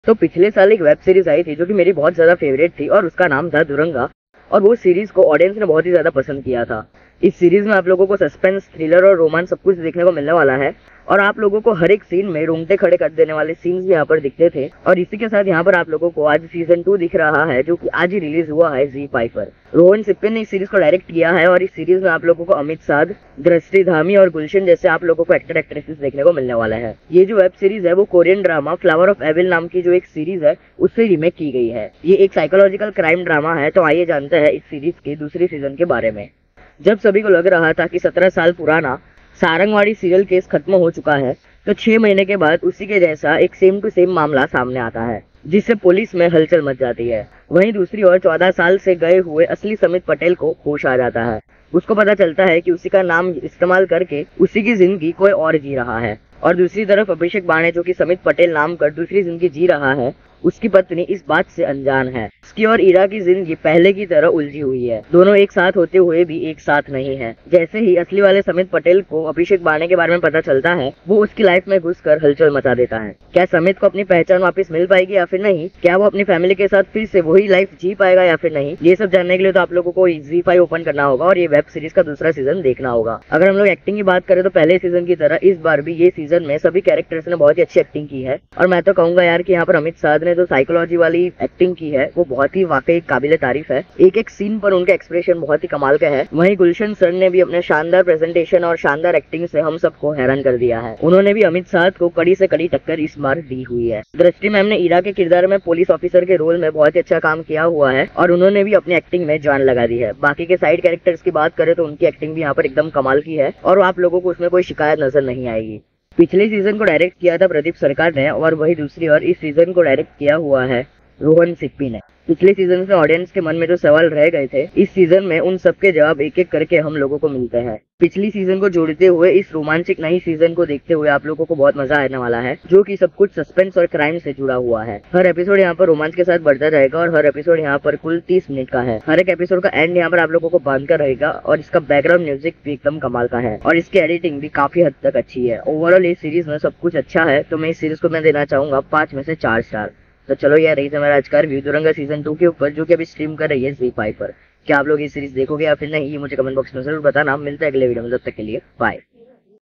Duranga Season 2 Review | Amit S | Gulshan D | Drashti D | A ZEE5 Original
Sammit is the ideal family man. What happens when his wife, inspector Ira's investigation into a series of murders leads her to him? Has Sammit been living the perfect life or the perfect lie?

'Duranga 2' picks up where the first season left off, with Sammit Patel waking up from a coma and setting out to regain his identity from Abhishek Bane, who has been living under his name. Simultaneously, Abhishek searches for his father's accomplice and confronts Sammit in a fierce battle of wits. The heart of this season lies in whether the truth behind their identities and past will be revealed.